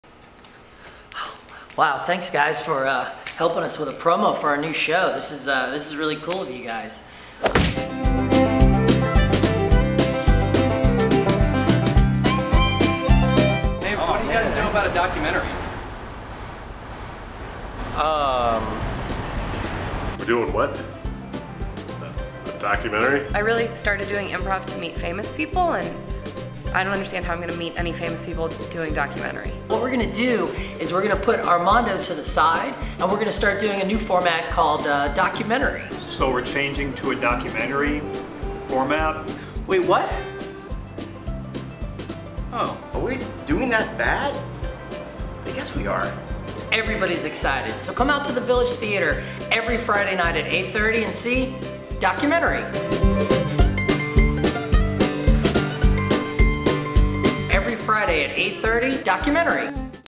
Documentary! Every Friday 8:30pm @ Village Theatre
Documentary is the Village Theatre's newest long-form improv show! It's now every Friday @ 8:30PM! Tickets are $10